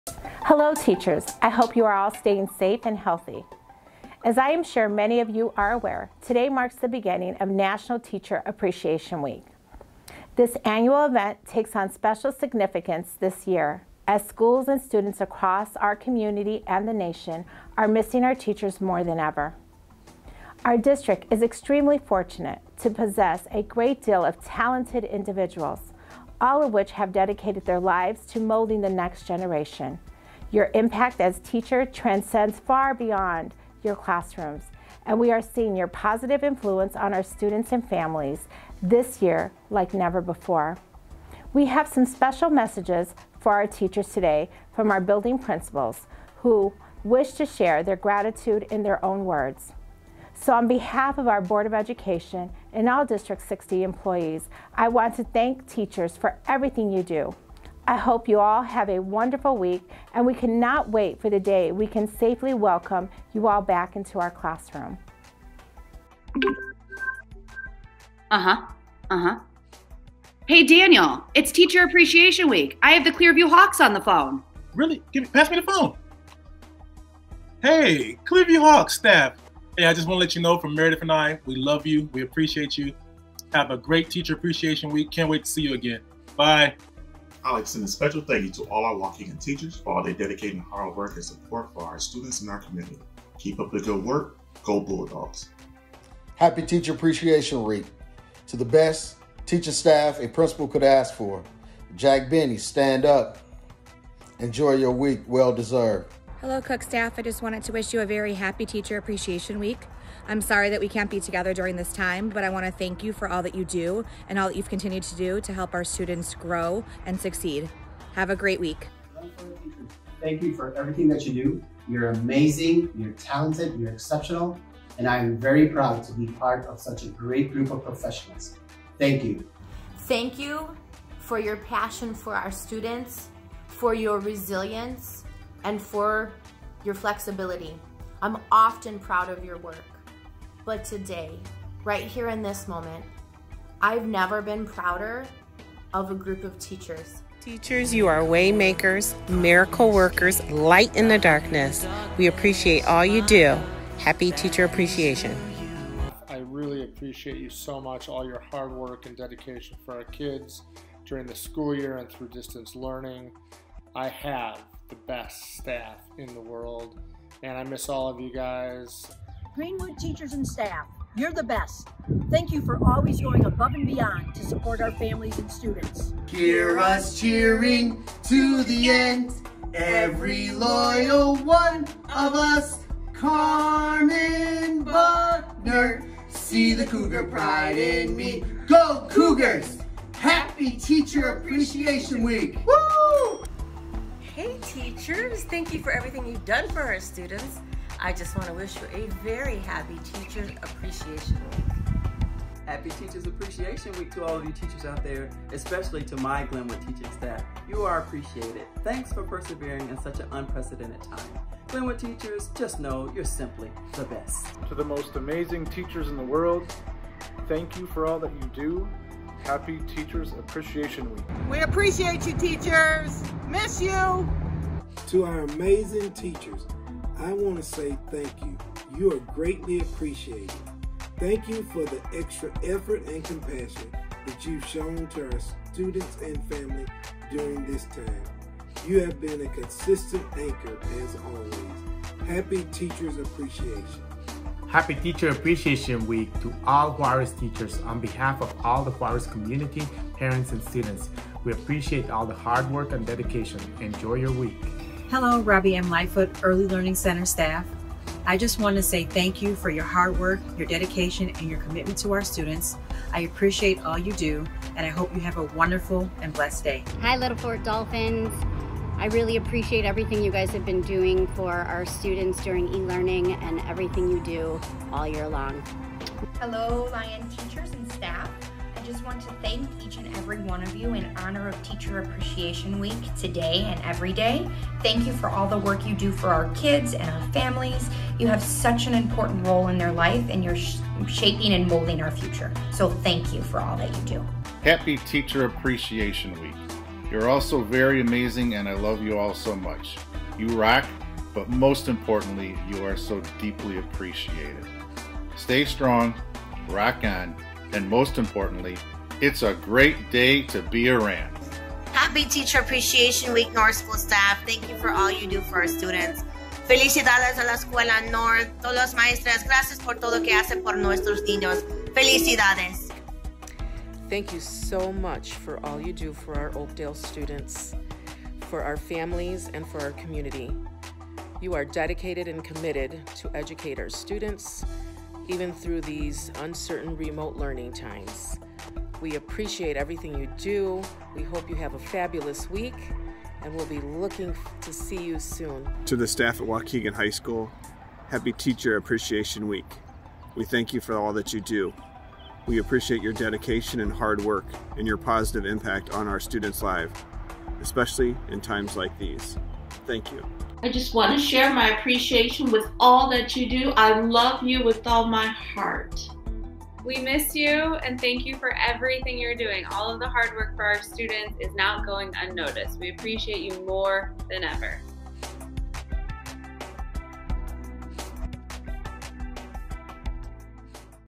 Waukegan Principals' Messages for Teacher Appreciation Week 2020
Waukegan Public Schools principals have special messages for teachers during Teacher Appreciation Week 2020.